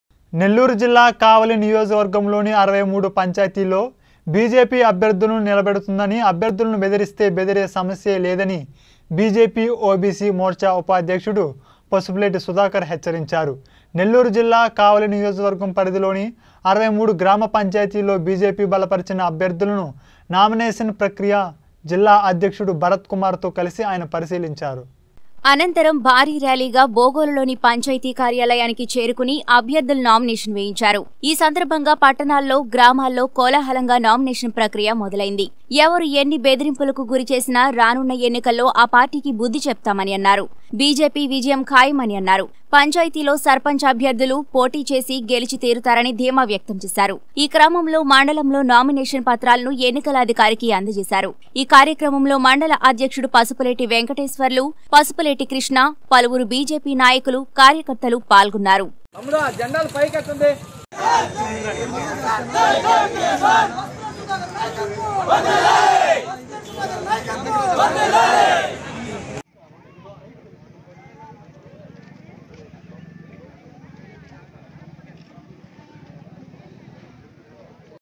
ఎవరు ఎన్ని బెదిరింపులకు గురి చేసిన రానున్న ఎన్నికల్లో ఆ పార్టీకి బుద్ధి చెప్తాం - పసుపులేటి సుధాకర్
ఎవరు ఎన్ని బెదిరింపులకు గురి చేసిన రానున్న ఎన్నికల్లో ఆ పార్టీకి బుద్ధి చెప్తాం బిజెపి ఓబీసీ మోర్చా ఉపాధ్యక్షుడు పసుపులేటి సుధాకర్ నెల్లూరు జిల్లా కావలి | 19 Newstelugu